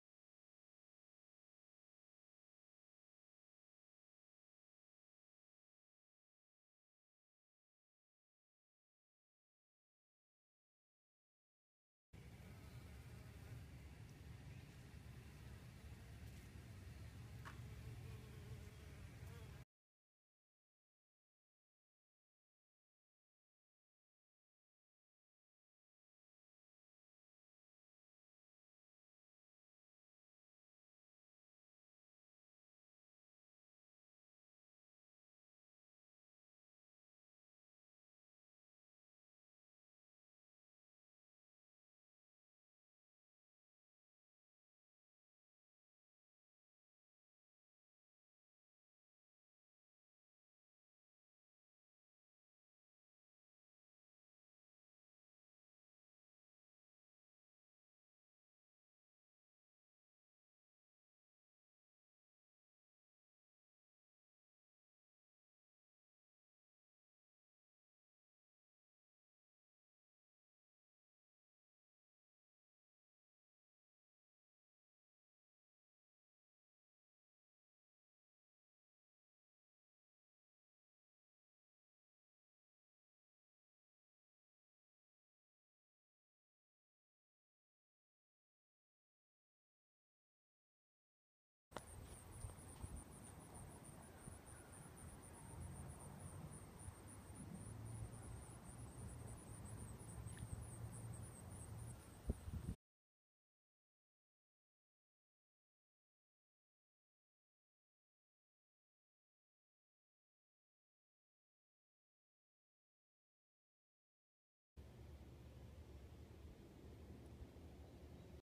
bees, monarch butterfles, dragonflies, frogs
bees, monarch butterflies, dragonflies, frogs in Minnesota Summer (June,July, August) 2015 The hive was near Peavy Lake in Morrison County (bees on there way to finding a new hive) the other photos and video are from Brainerd MN in Kiwanis Park (monarch near Boom Lake) and community garden (bees and frogs).